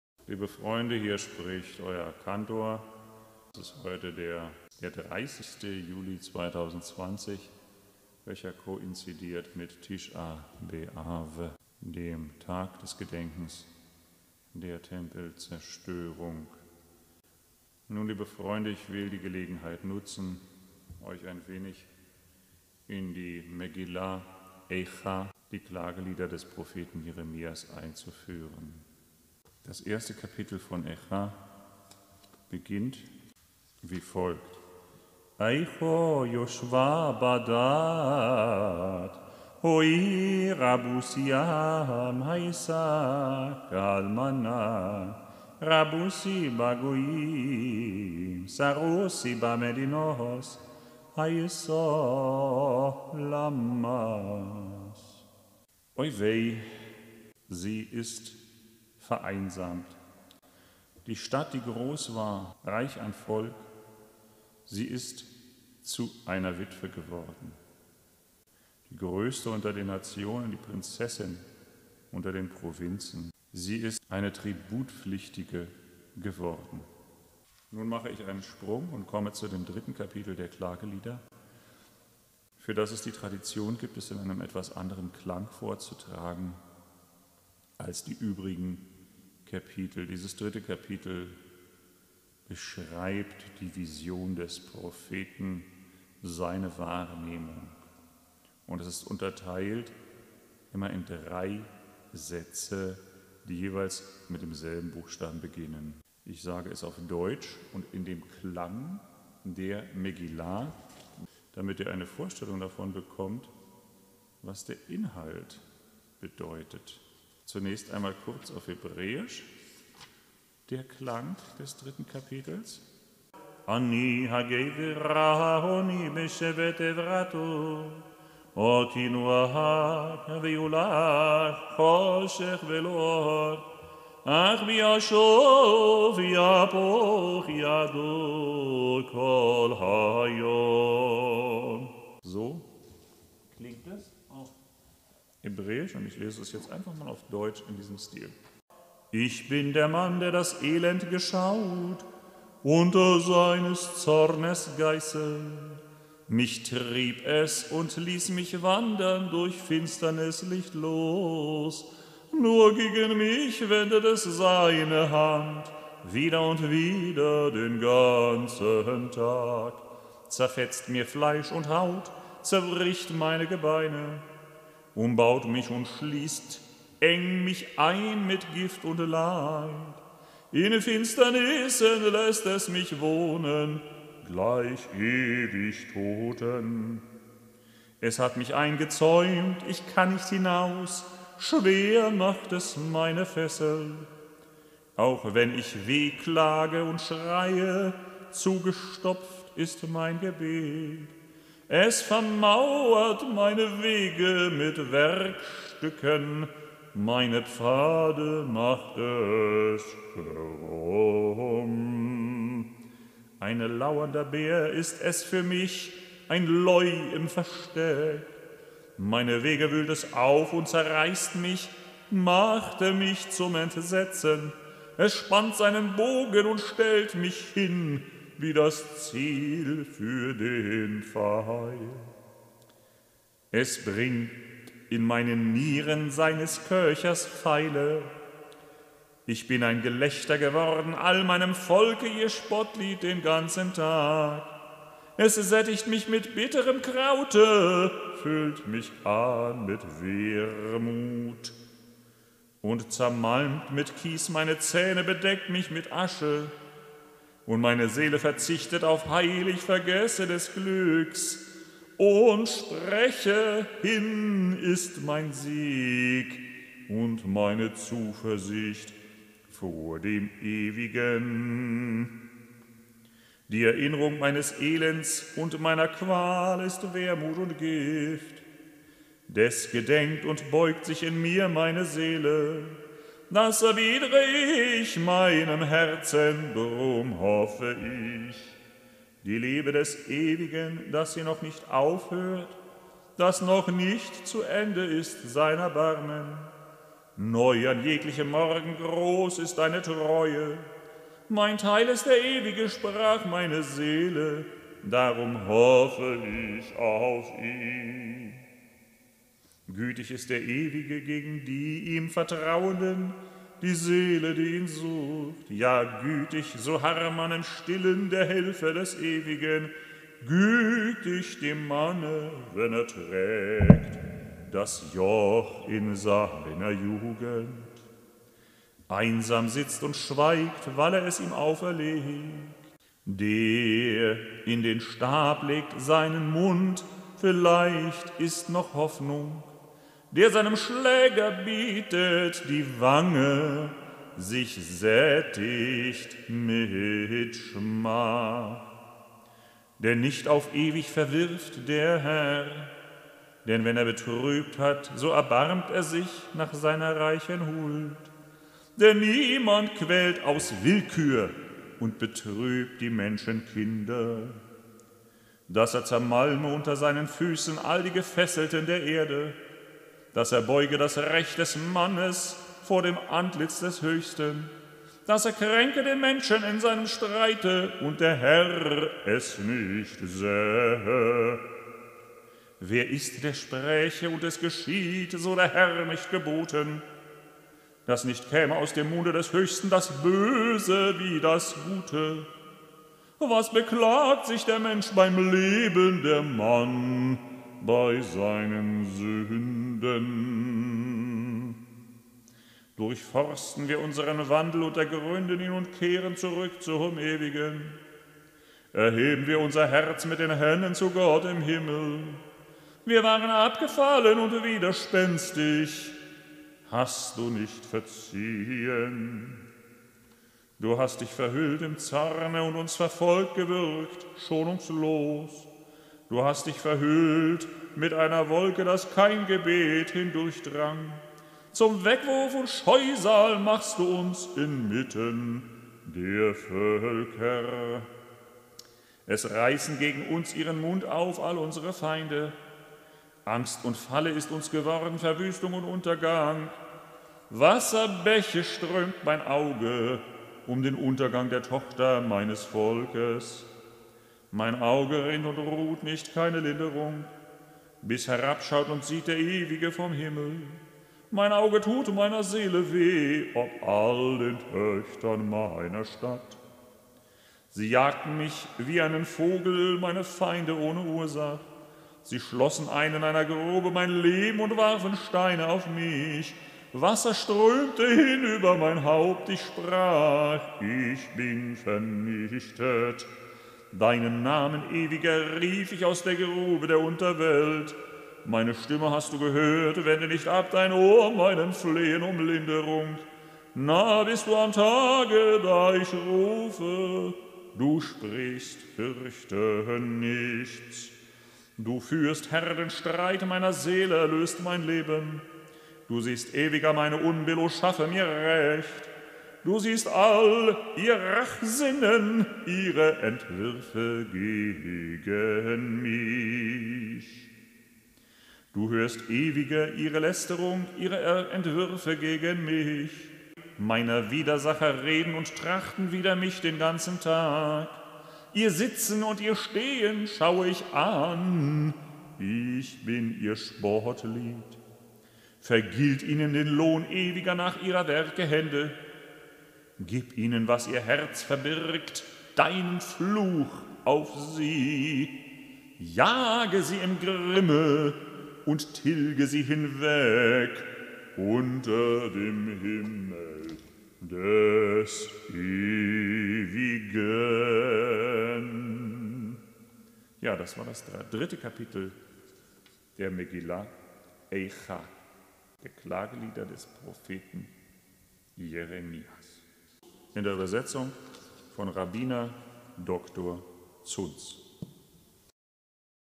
Eichah Kapitel 3 auf Deutsch im Nussach
Chapter 3 of Megillat Eichah. German translated text  begins at 2.30!